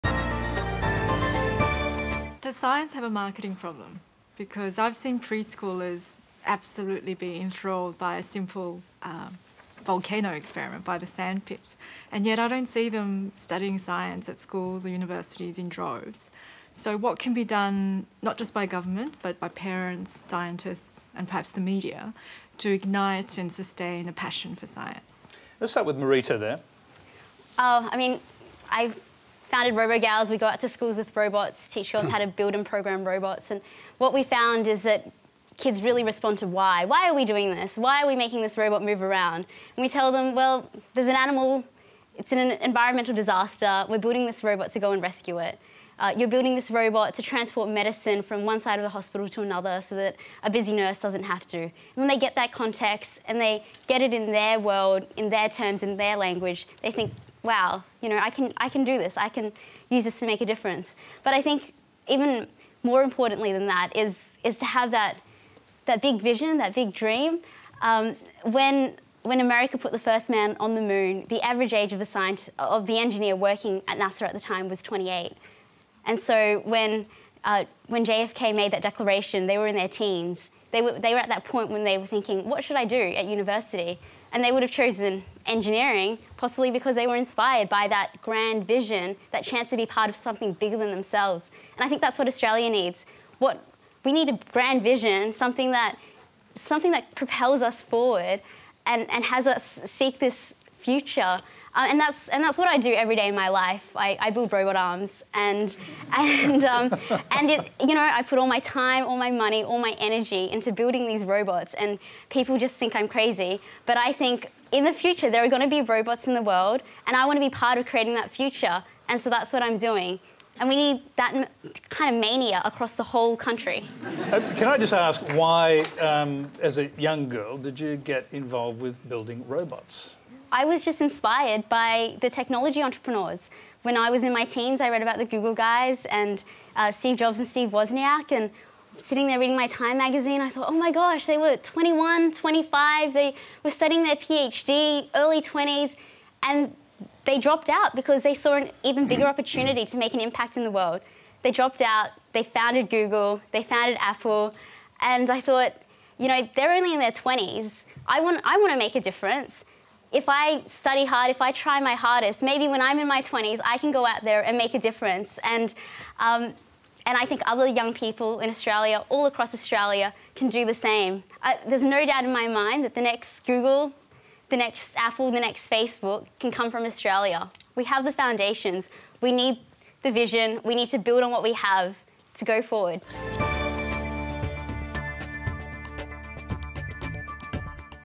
Q&A Highlight – Does science have a marketing problem?
Marita Cheng talks about teaching children about robotics and how we should be working towards inspiring the next generation of scientists and engineers.